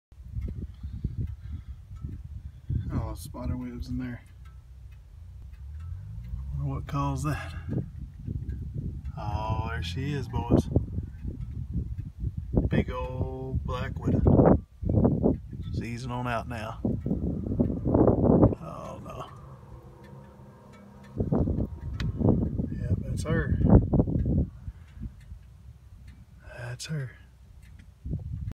Black Widow Spider In Electric Box
Black Widow Spider(AKA Latrodectus) hiding in an electric box in the Oilfield.

God Bless.